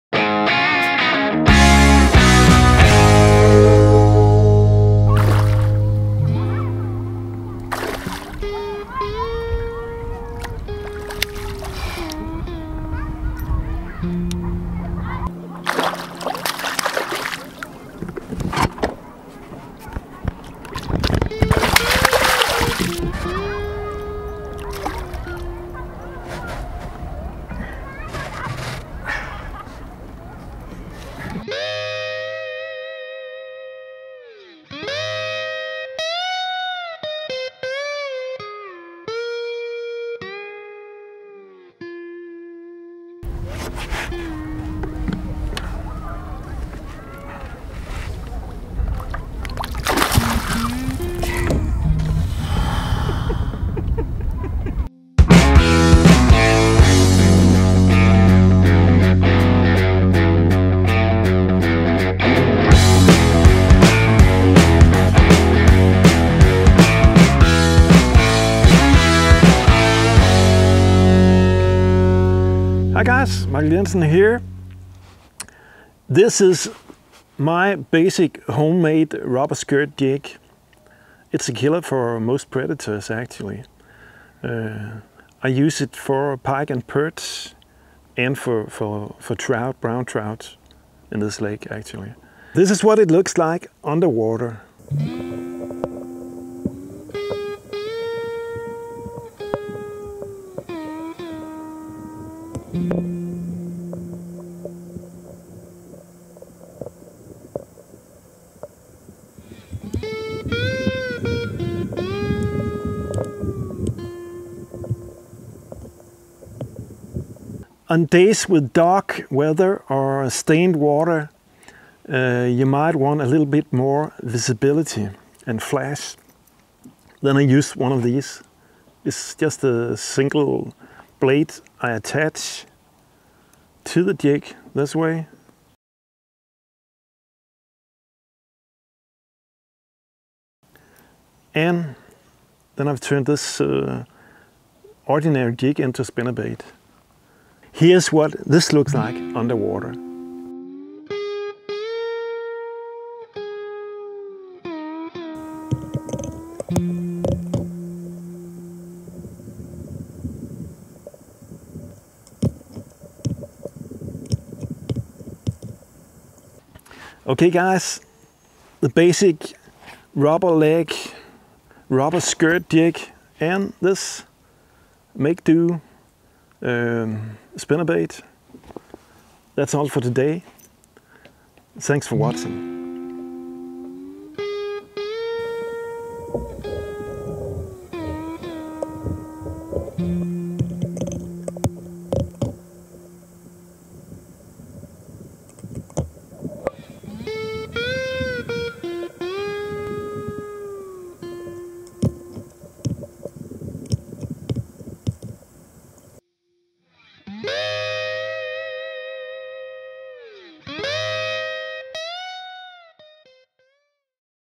Turn your jig into a spinnerbait - lure action underwater footage
How to turn your favourite jig into a spinnerbait, using a Lindy jig spinner. The blade adds more flash, commotion and sound to your jig - which can be a bonus in dark weather conditions, in muddy or stained water or during night fishing. This combo is deadly for pike, perch, walleye, musky, zander, bass and large trout.